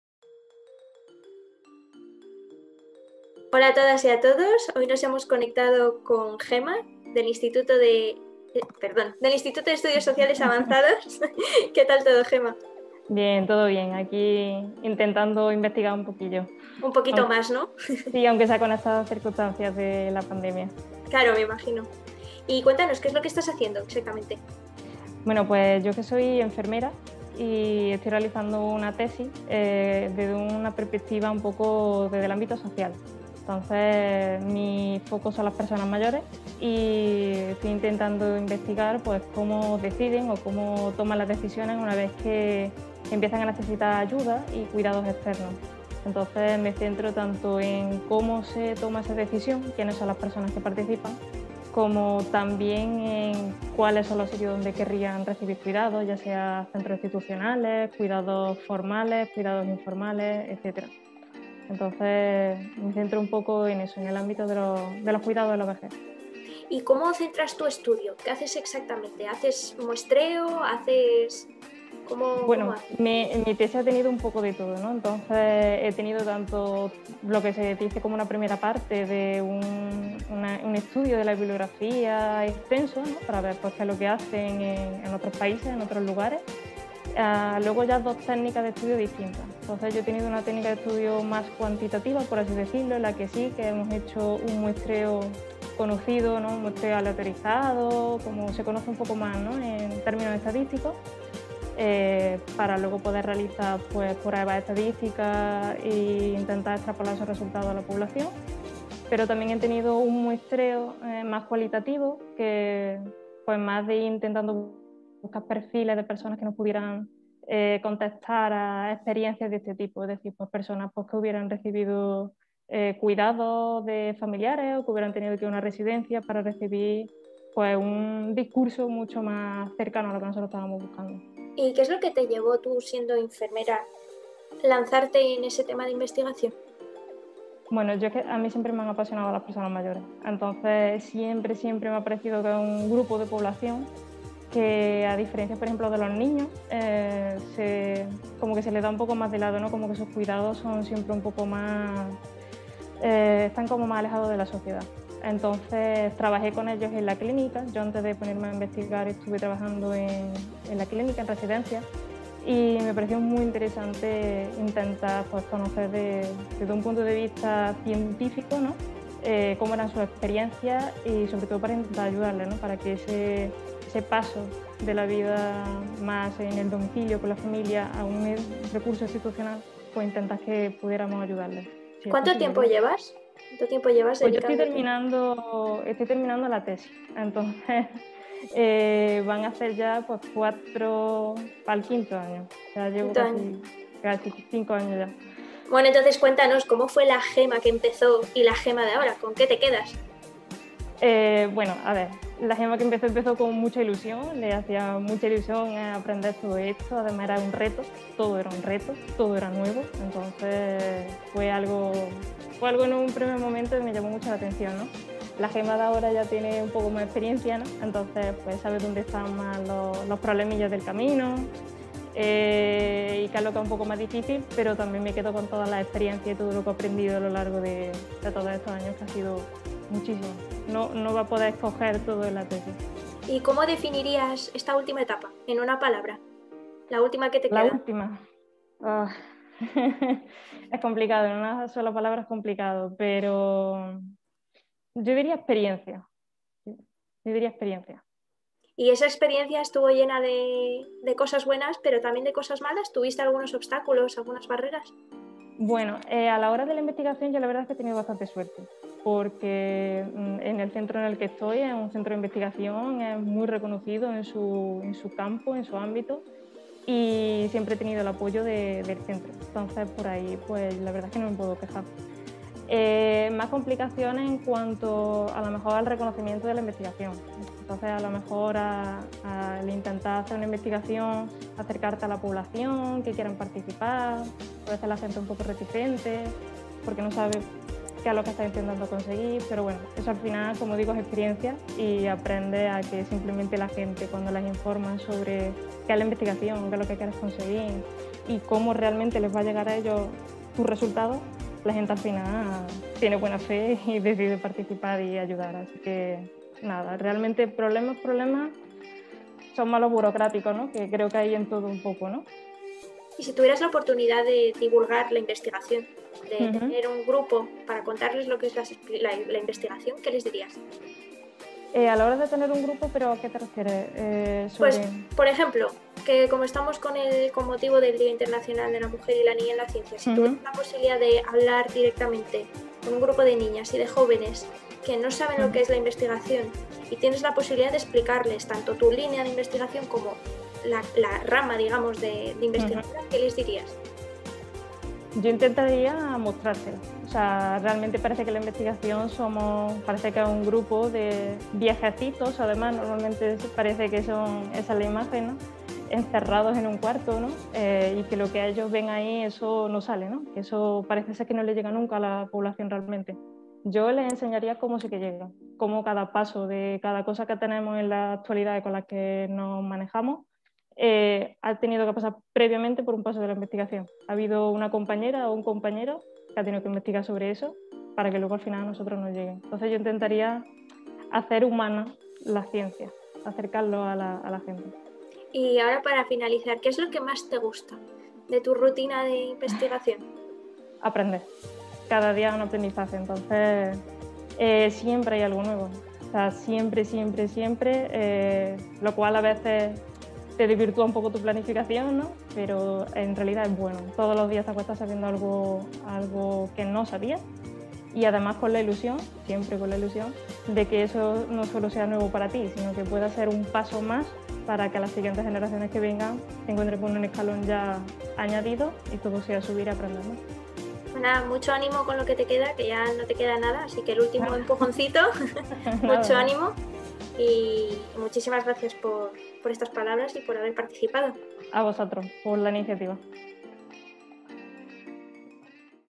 Gema Serrano Gemes - Entrevista 11F
Gema Serrano Gemes es del Instituto de Estudios Sociales Avanzados (IESA-CSIC).
Su tesis aborda la difícil decisión a la que se tienen que enfrentar las personas mayores cuando empiezan a necesitar ayuda, y es: ¿Dónde y por quién quieren ser cuidados? Está estudiando quiénes son los participantes en esta toma de decisiones, las dinámicas que se generan, las opciones elegidas/preferidas, así como las posibilidades de mejora que como sociedad podemos plantearnos.